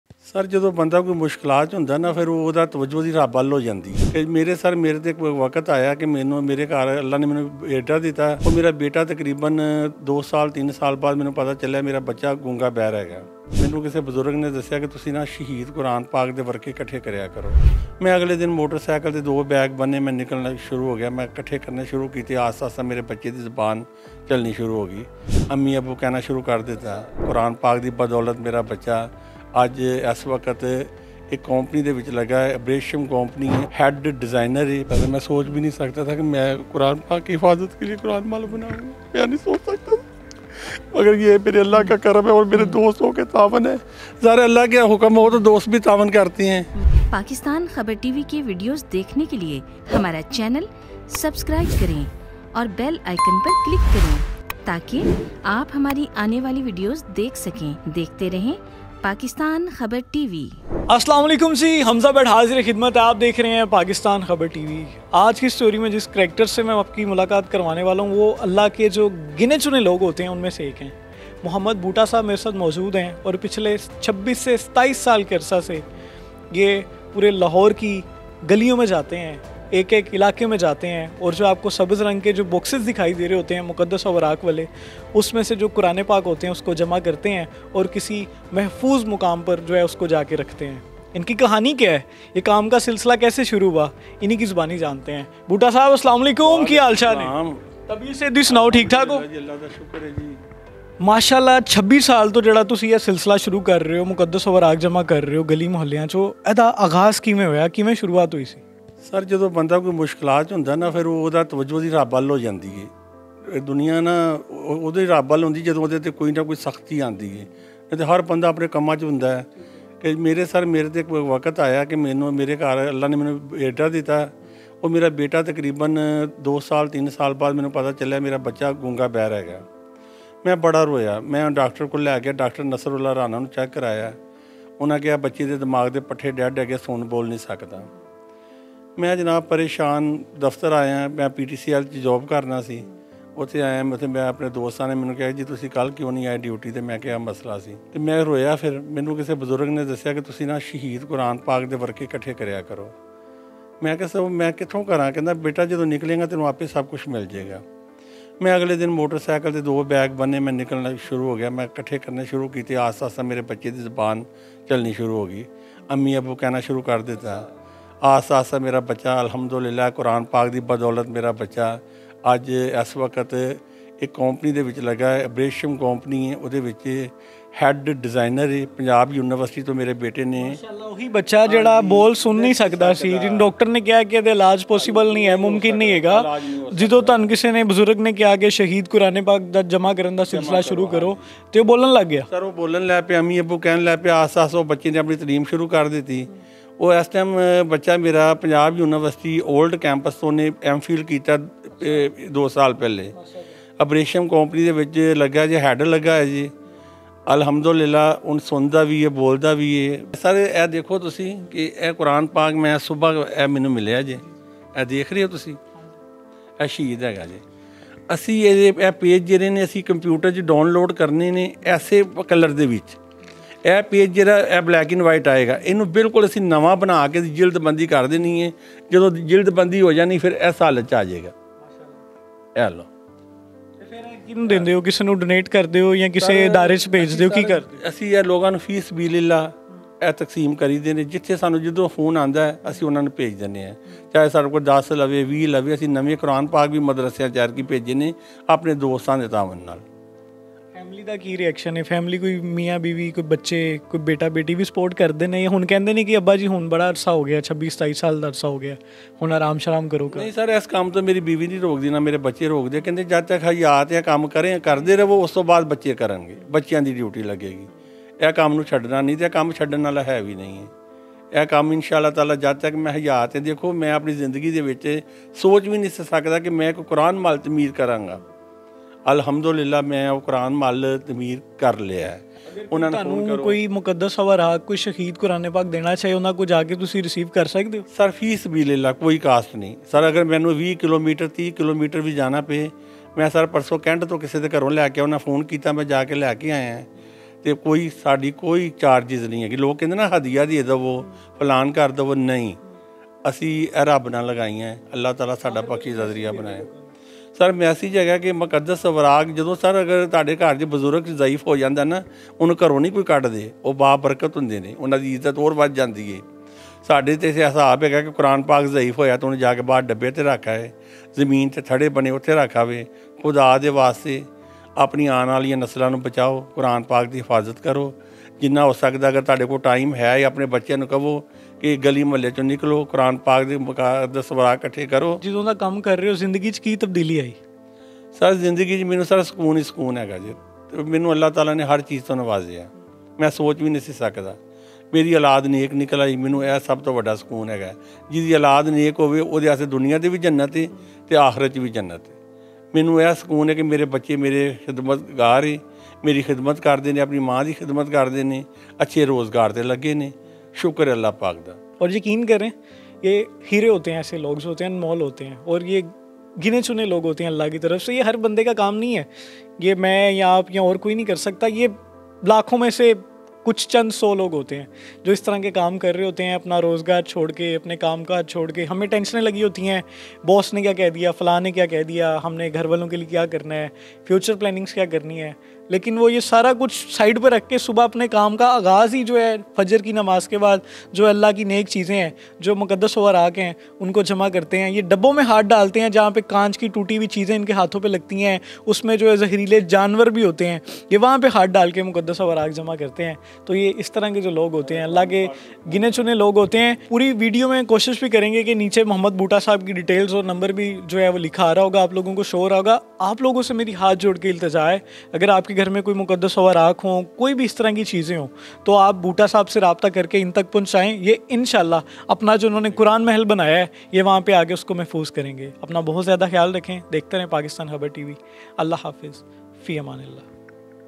26 Saal sy Muqadas Oraq IKathy Kerny Wala Bazurg | QURAN PAK Mehfooz Kerny Ky Liye Mehal Bana Dia
26 Saal sy Muqadas Oraq IKathy Kerny Wala Bazurg | QURAN PAK Mehfooz Kerny Ky Liye Mehal Bana Dia

Witness the incredible dedication of Barzag, who has been collecting holy papers for 26 years. Discover the story behind the palace built to preserve these revered Quranic manuscripts. Join us as we delve into the profound significance of this sacred endeavor.